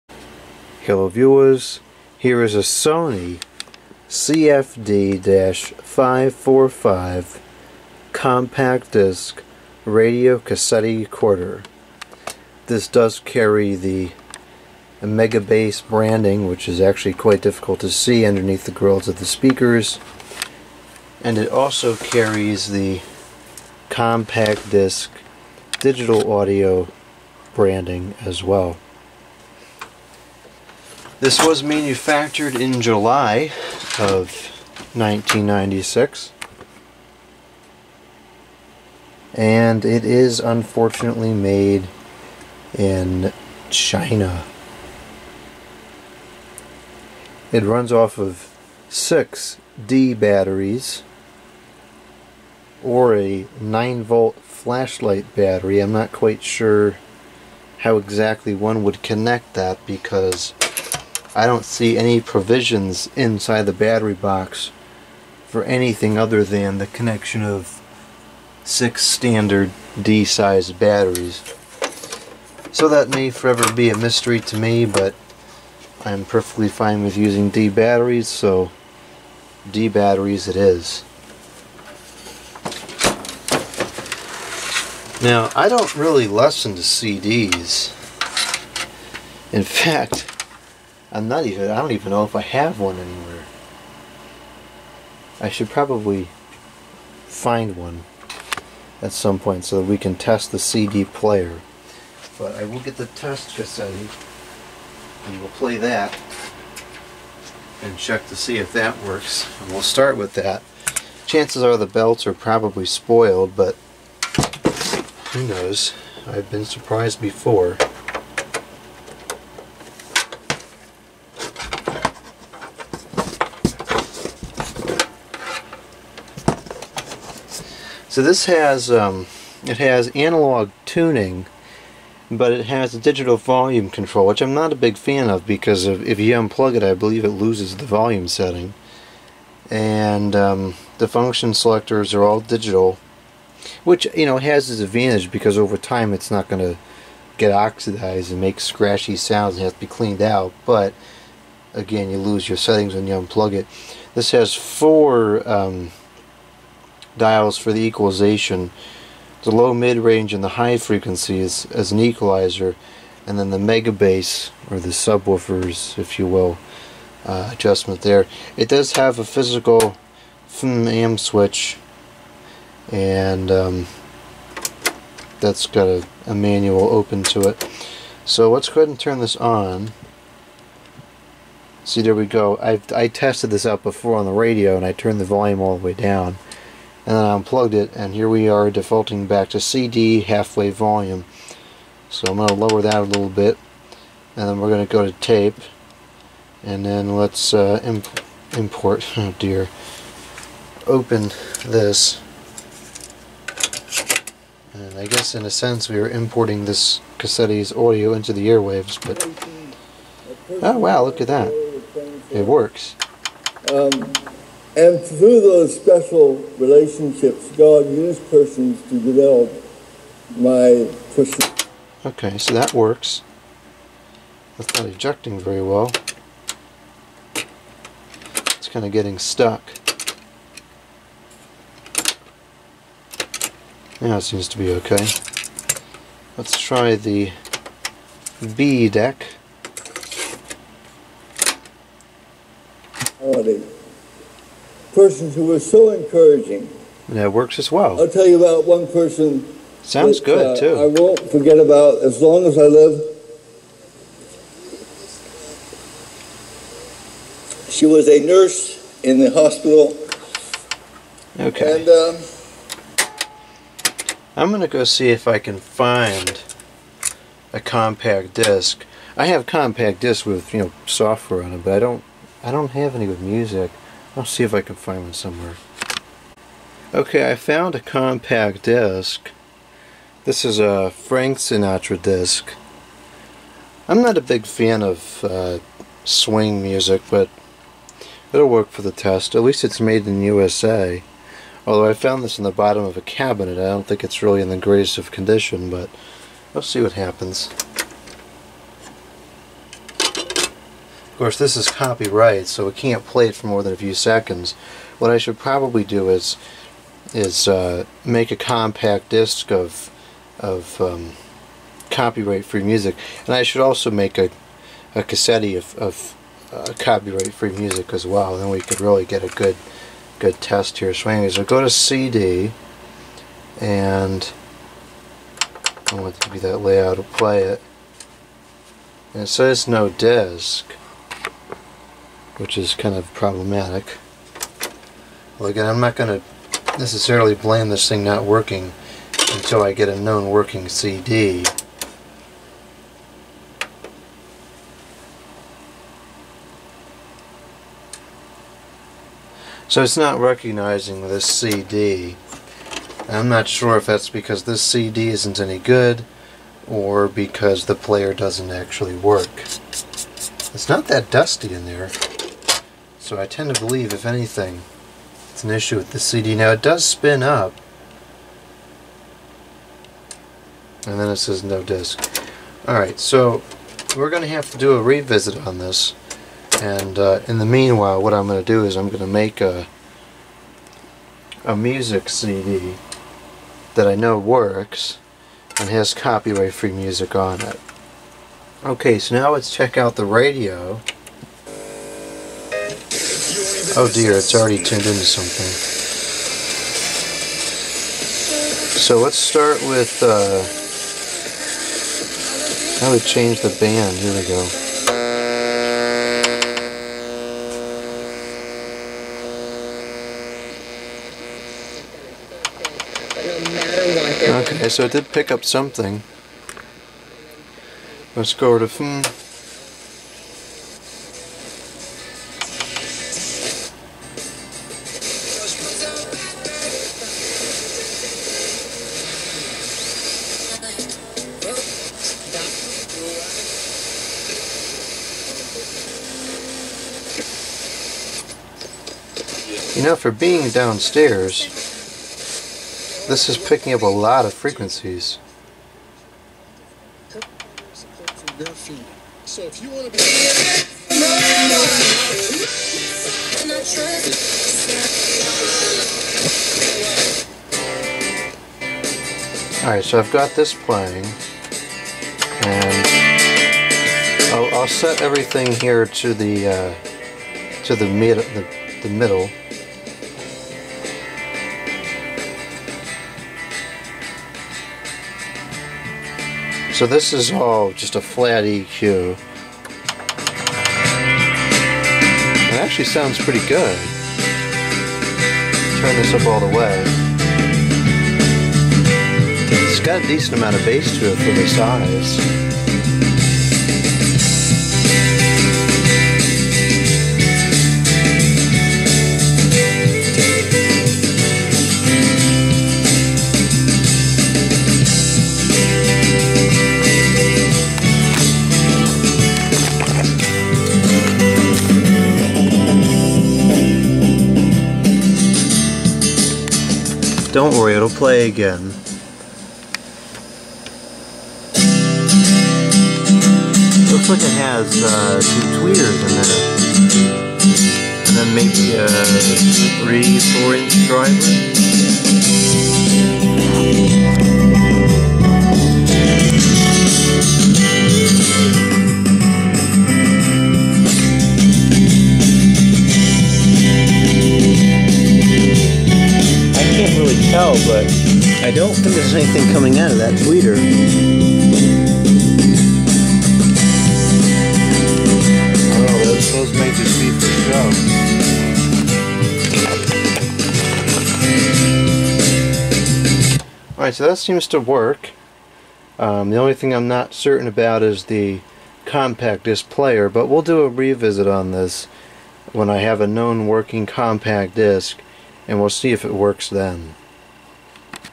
Sony CFD-545 Boombox | Initial Checkout
The initial checkout of a Sony boombox, model CFD-545..